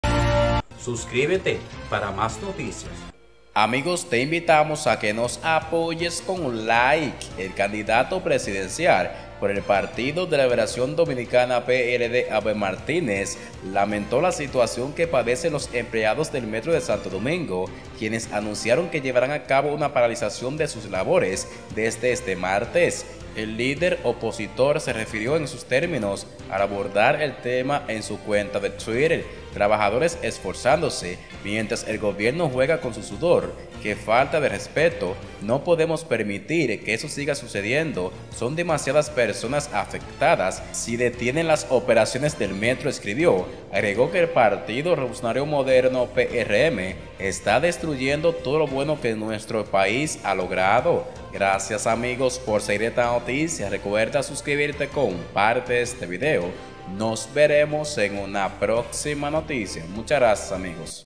LO QUE LE DIJO EL CANDIDATO ABEL MARTÍNEZ AL PRESIDENTE LUIS ABINADER
El candidato presidencial por el Partido de la Liberación Dominicana (PLD), Abel Martínez Durán, lamentó este lunes la situación que padecen los empleados del Metro de Santo Domingo, quienes anunciaron que llevarán a cabo una paralización de sus labores desde este martes.

El líder opositor se refirió en esos términos al abordar el tema en su cuenta de Twitter.

«Trabajadores esforzándose, mientras el Gobierno juega con su sudor. ¡Que falta de respeto! No podemos permitir que esto siga sucediendo. Son demasiadas personas afectadas, si detienen las operaciones del metro», escribió.

Agregó que el Partido Revolucionario Moderno (PRM), está destruyendo todo lo bueno que nuestro país ha logrado.

¡El país necesita un gerente!», concluyó.